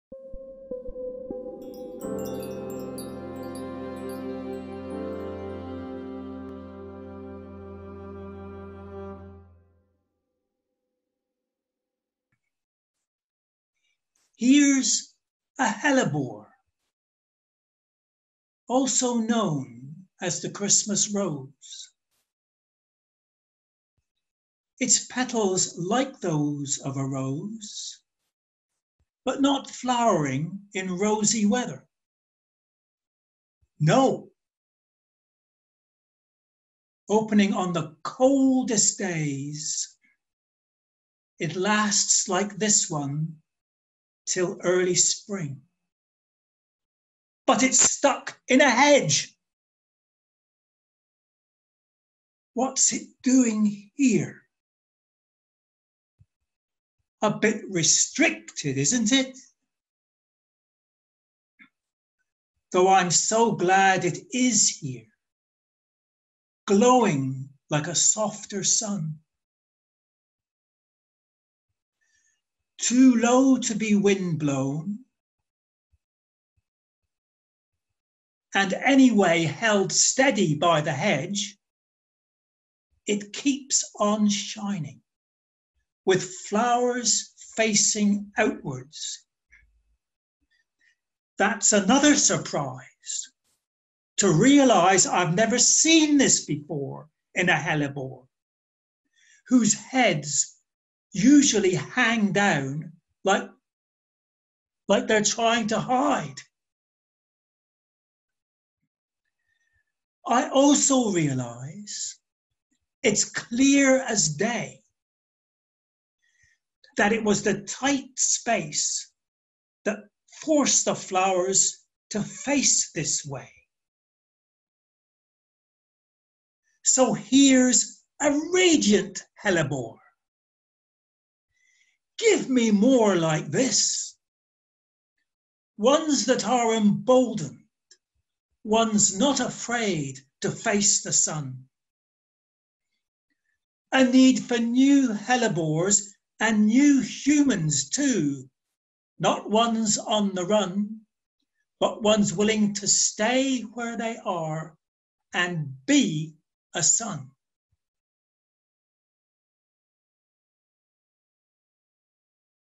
Brian Perry Reads 'Wanted: More Suns!'
September 6, 2020: Friend of Sunrise Ranch Brian Perry reads an original poem called "Wanted: More Suns!" during the Sunday service.
 Brian was speaking from his home in the United Kingdom.

SunriseRanch.org